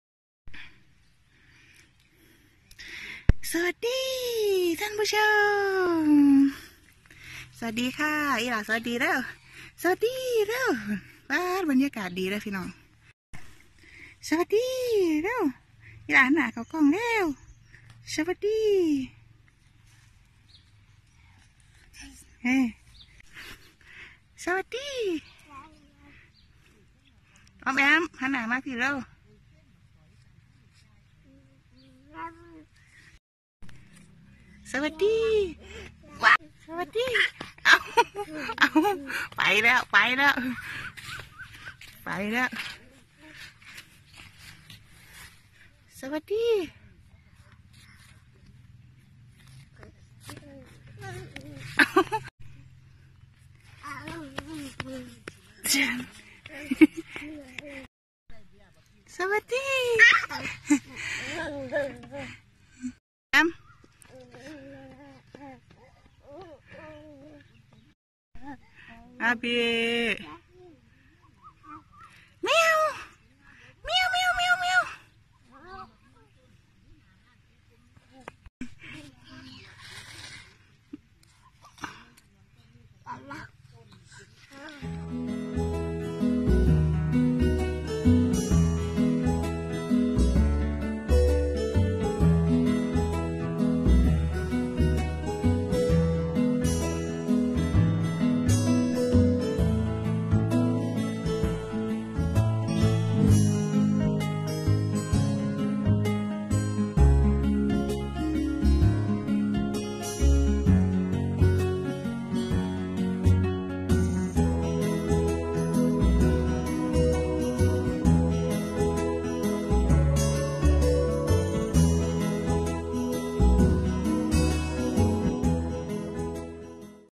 เด็ก​น้อย​บ้านนอก​เล่นขี้ดินกะบ่เป็นหยังยิ่งกล้าเลอะยิ่งเยอะประสบการณ์​
อ๋อมแอ๋​ม​ 10​ เดือน​ 23​ วัน​